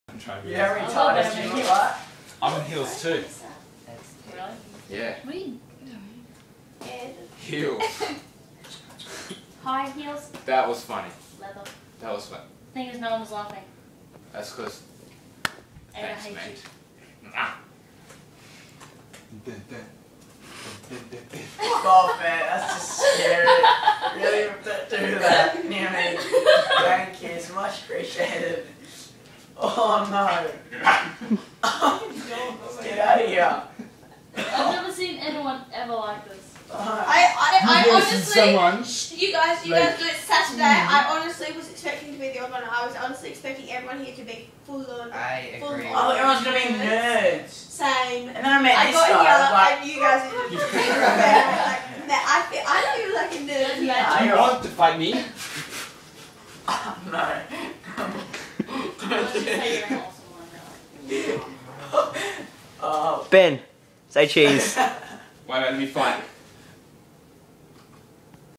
Sir Charles Court Young Leaders 2011 Ben...
Ben Robertson from Christmas Island. 2011 good times and us acting as leaders :)